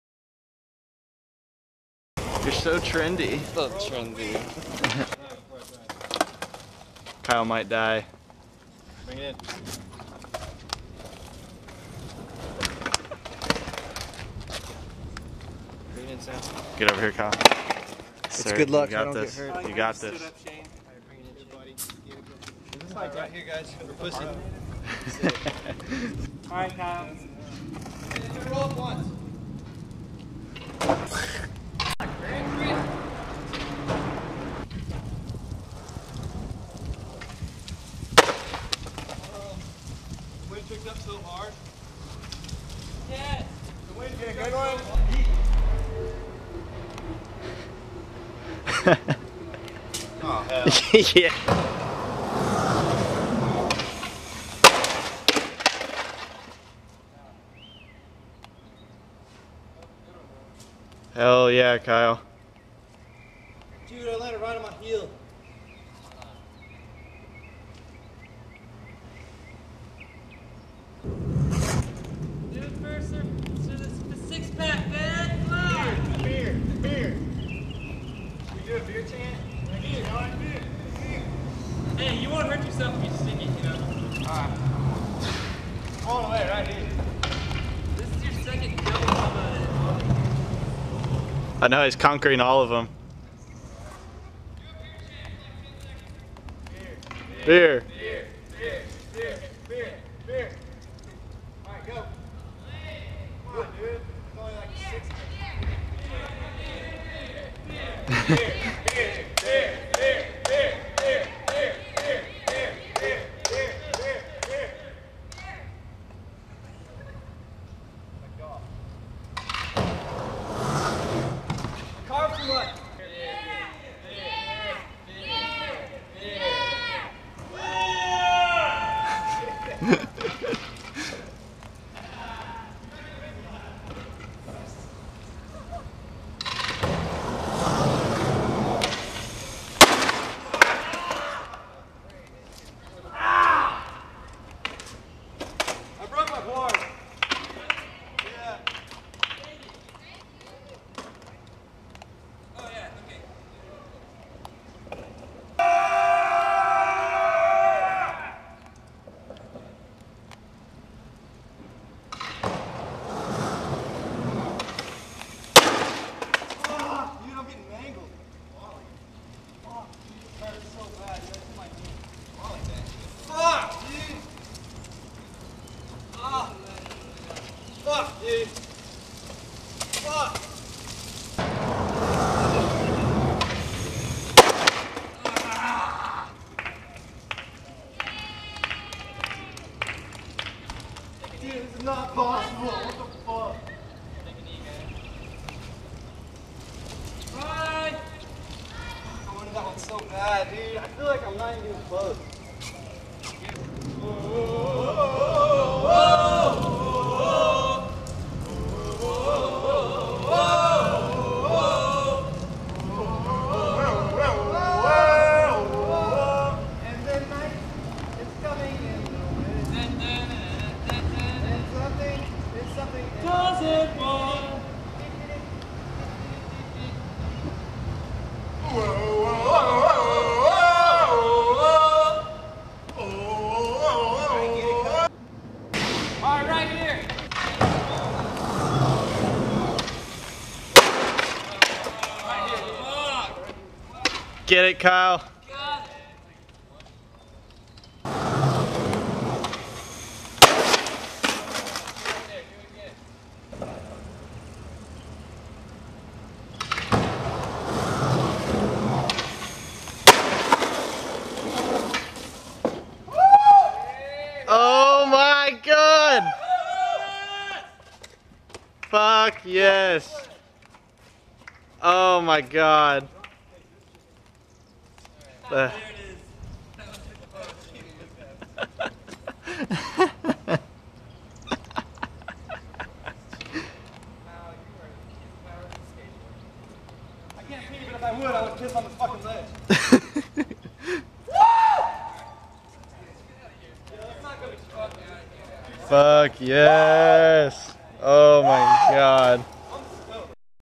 Kyle Stone at Fair Park
Behind the scenes of a banger Kyle Stone laid down a few months back with Bret Gregory, Kris Kenendy, Anthony Padilla, Blair Trammell, and Shane Darnell.

Filmed by Sam Laceky